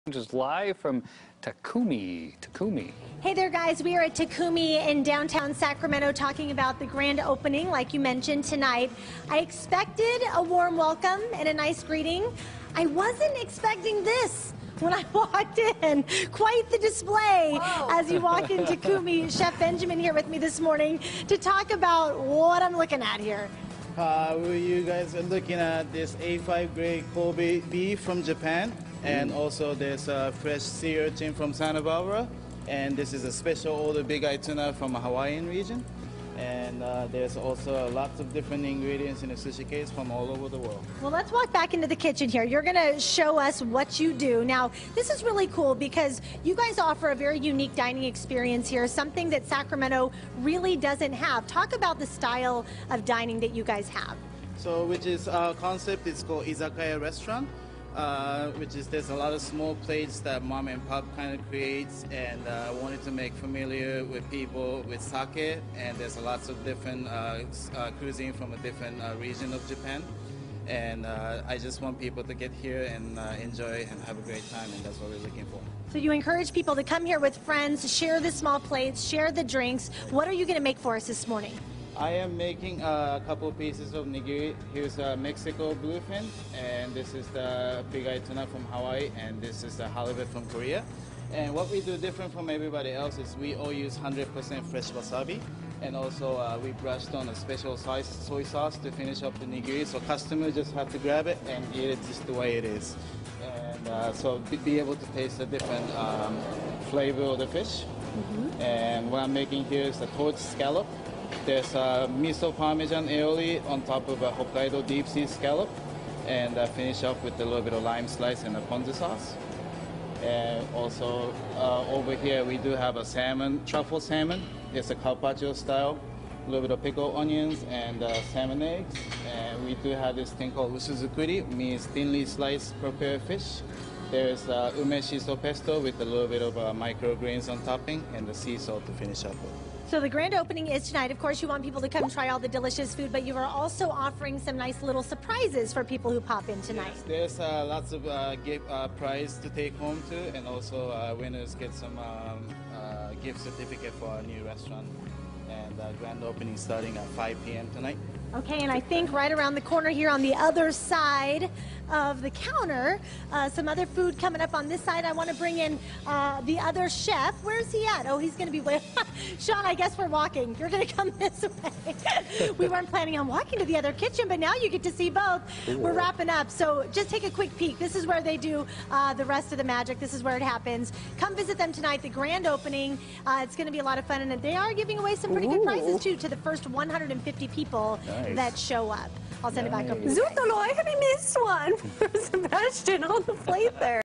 Takumi Grand Opening
Today we are at the grand opening for a new Japanese restaurant to see what dishes they serve.